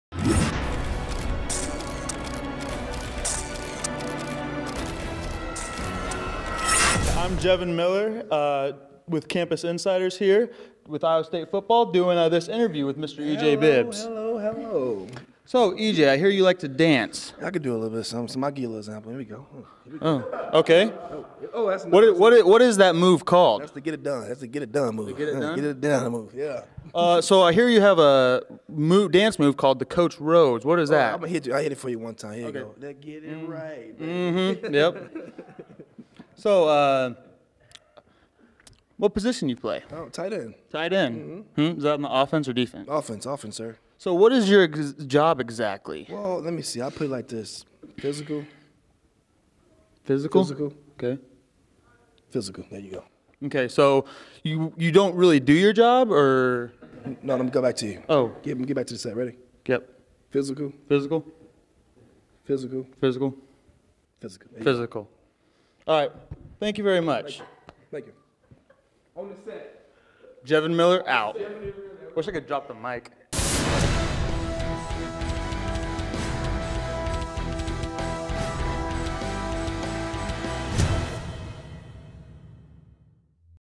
Iowa State Cyclones Take The Microphone | CampusInsiders
Linebacker Jevohn Miller interviews tight end E.J. Bibbs about his moves on and off the field.